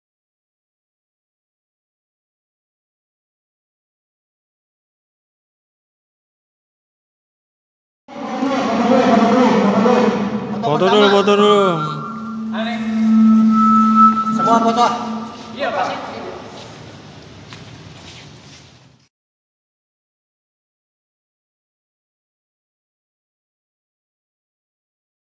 Table tennis, penyerahan hadiah pemenang pertandingan internal komunitas PTMSI Cimahi 12-12-2020
Penyerahan hadiah juara pertandingan pingpong internal komunitas PTMSI Cimahi yang diselenggarakan tanggal 12 Desember 2020.
Juara 4 pa Hasan, juara 3 pa Sumadi, juara 2 pa Asep, juara 1 pa Radik.